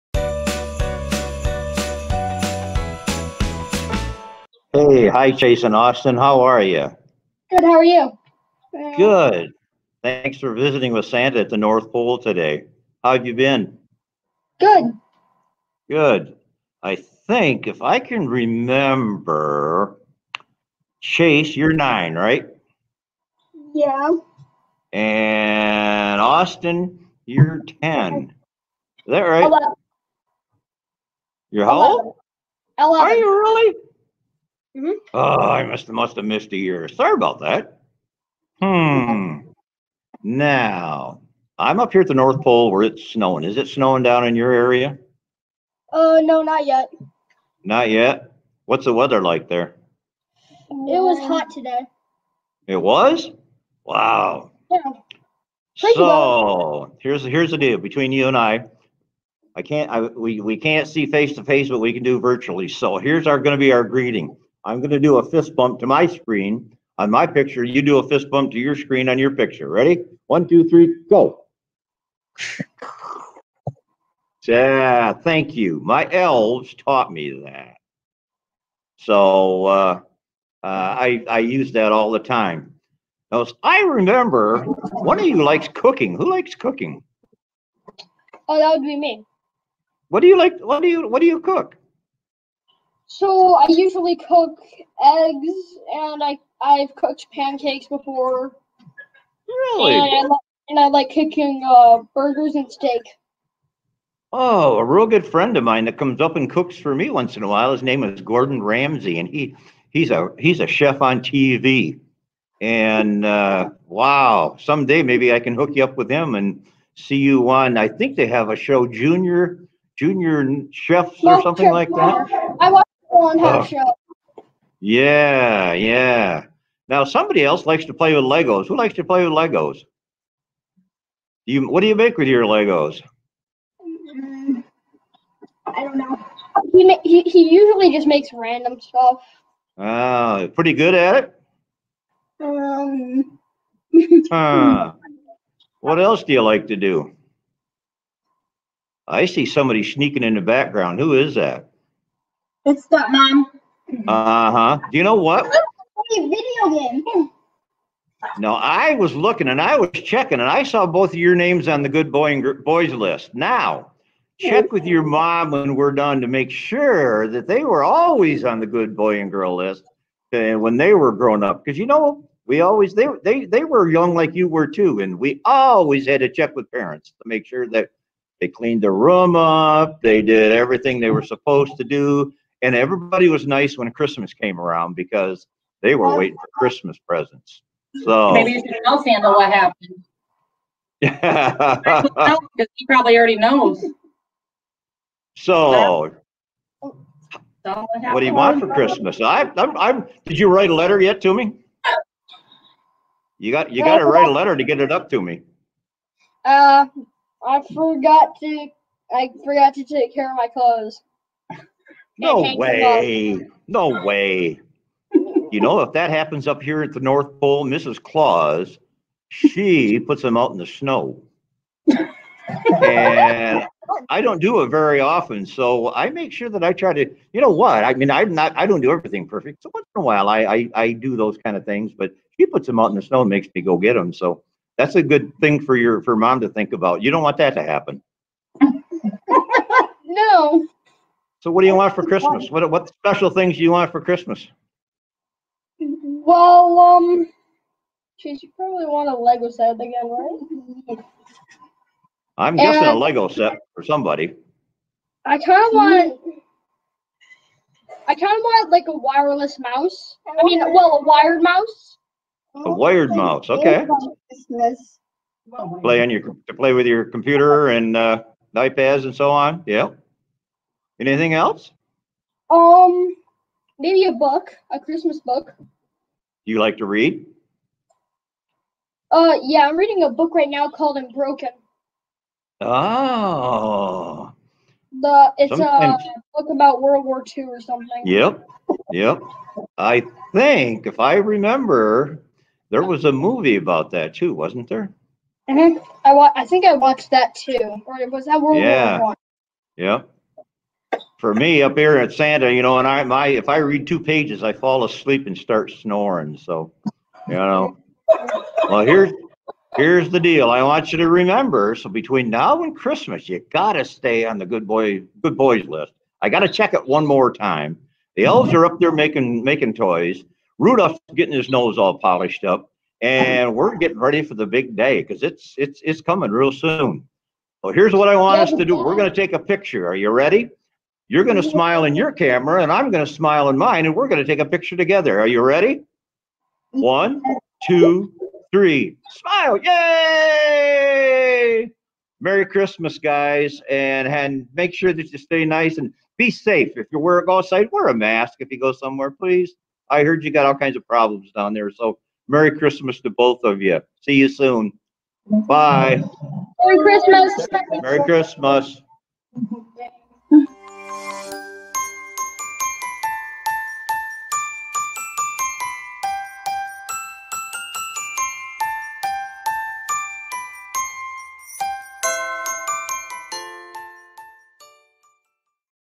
Virtual Visit With Santa - Chase & Austin
Santa visits with Chase & Austin in celebration of the holidays